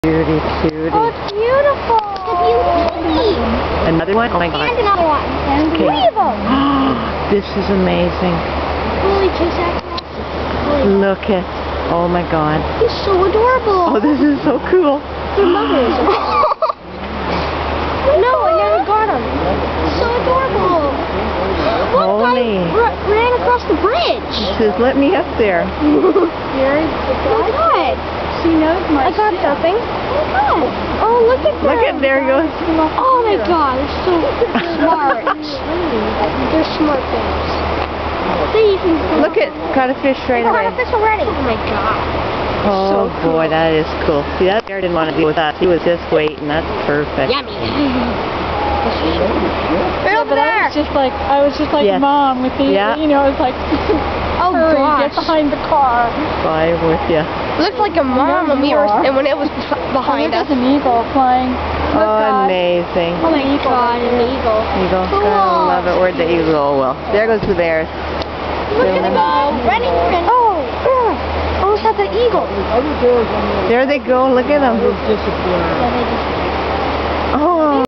The Three Bears in Haines, Alaska
Every evening, on the Chilkat River, the grizzlies come out to fish.  We were standing beside the river watching a mother and 2 cubs when we heard some sounds in the forest beside us.  We looked and saw a snout emerge, check to see that we were not in its path and then out came 3 bears.